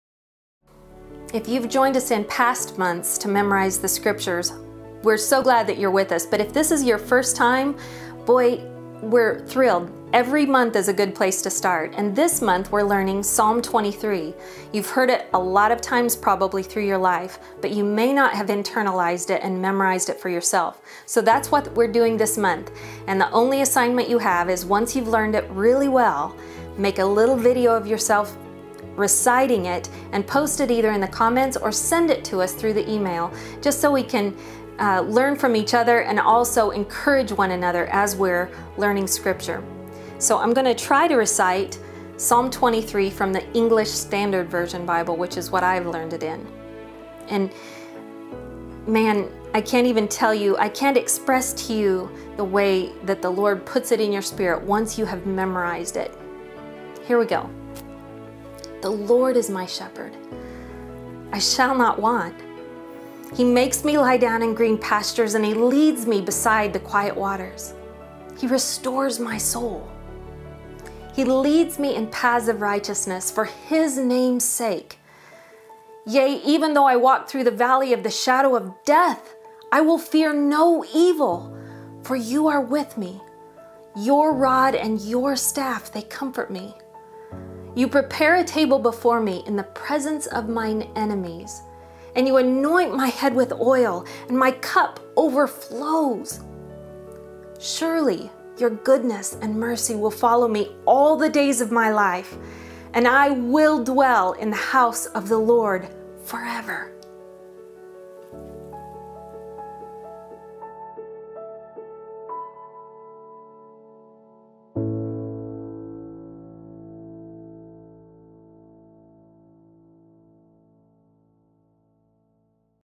PSALM 23 - Memorize in a Month! -with PREPSTEADERS
Each Month we memorize a Chapter from the Holy Word of God, the Bible. This month we are learning Psalm 23 (I use the ESV Version.)  Learn it with us, and once you have it down cold, record a video of yourself reciting it and post it below!  Hearing and seeing each other learn scripture together helps it sink deeper into our hearts and it changes us in the very best of ways! :)  There is no better way to be Ready and Wise!

PSALM 23 (English Standard Version)

1 The Lord is my shepherd; I shall not want.
2 He makes me lie down in green pastures.
He leads me beside still waters.
3 He restores my soul.
He leads me in paths of righteousness
for his name's sake.
4 Even though I walk through the valley of the shadow of death,
I will fear no evil,
for you are with me;
your rod and your staff,
they comfort me.
5 You prepare a table before me
in the presence of my enemies;
you anoint my head with oil;
my cup overflows.
and I shall dwell in the house of the Lord
forever.

Much more info over at PREPSTEADERS.com
Learn from the Past. Prepare for the Future.

Special thanks to ChristosFilms.com for filming & editing our videos!

Panasonic DMC-GH4 Camera we use